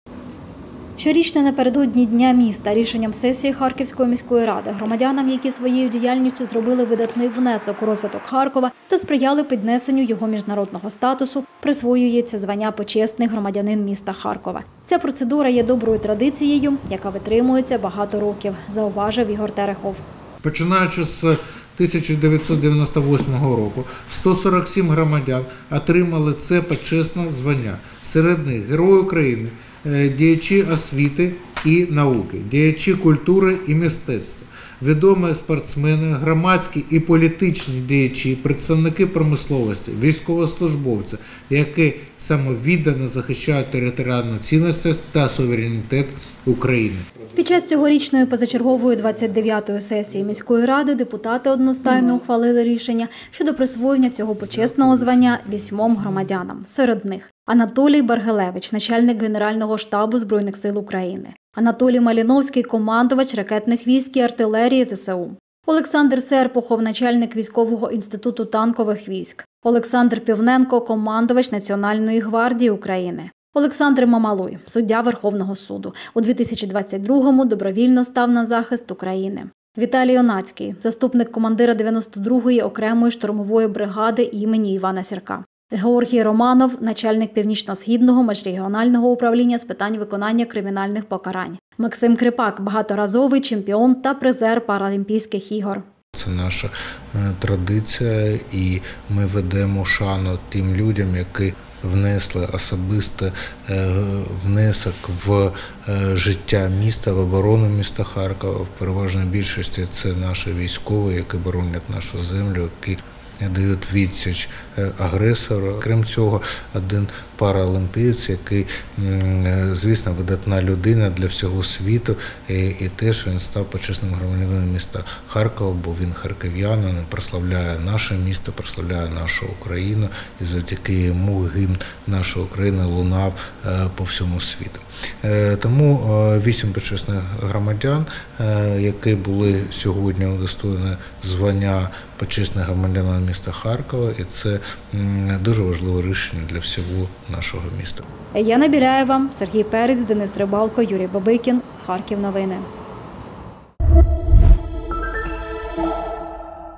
Обрано харків'ян, які отримали звання "Почесний громадянин Харкова"
Звання "Почесний громадянин міста Харкова" присвоєно восьмерим  харків'янам на позачерговій 29 сесії Харківської міської ради восьмого скликання. Хто носитиме цей визначний титул - в сюжеті.